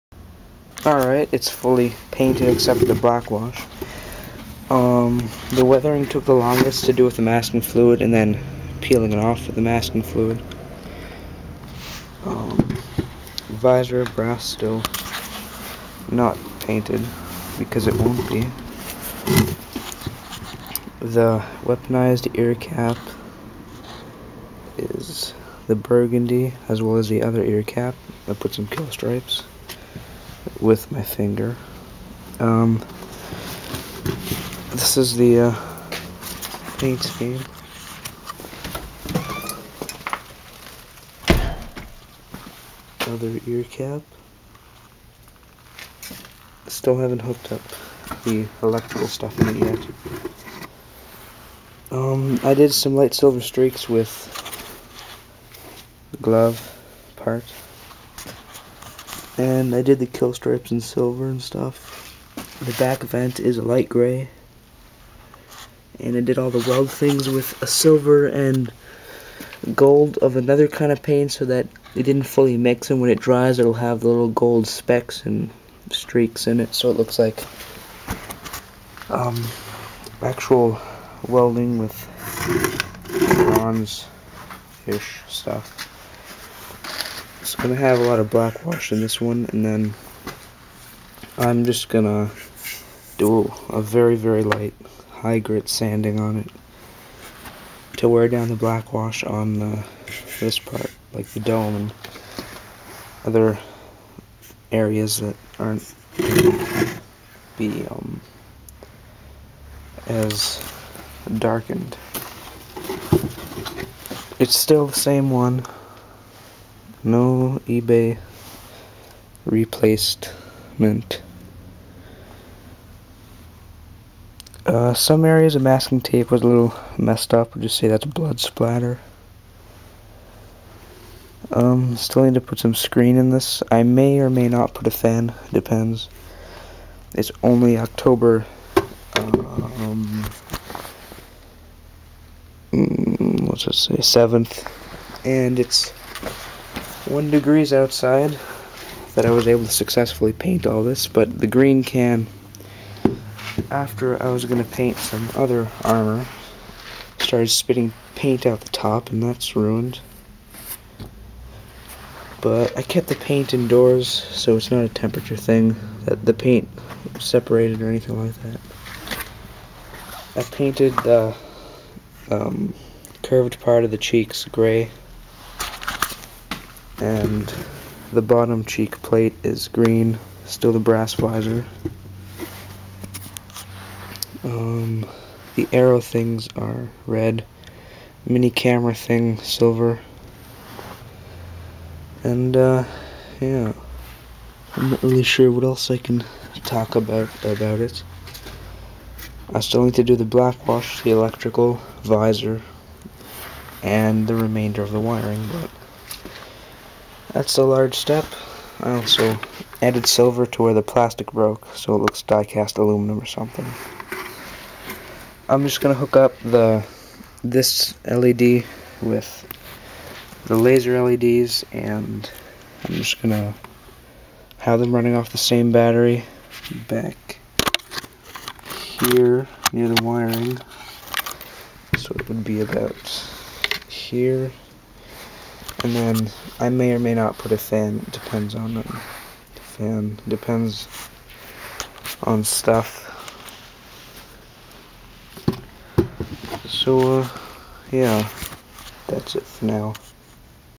Metal Mandalorian Helmat part 7
its painted and electronics worked on